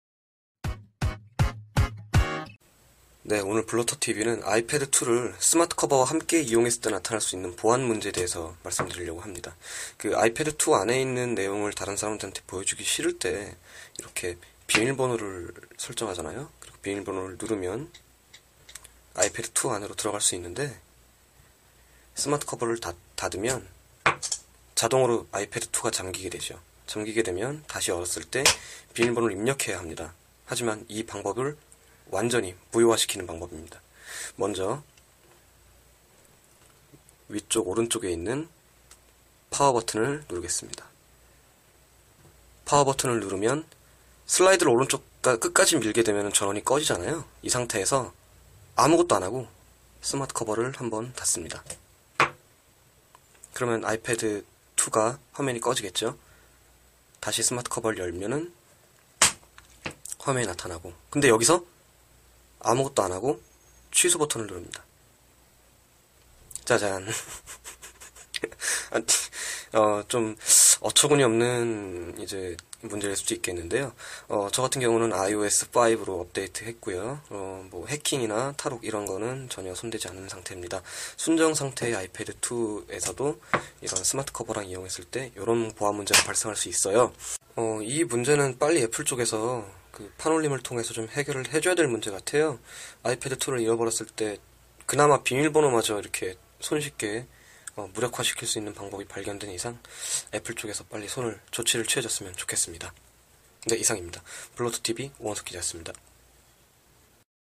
iPad2 security flaw with smart cover
iPad2 Security flaw with Smart cover: How to unlock the user password on iPad2 with Smart cover, Made by Bloter.net, Korea, Seoul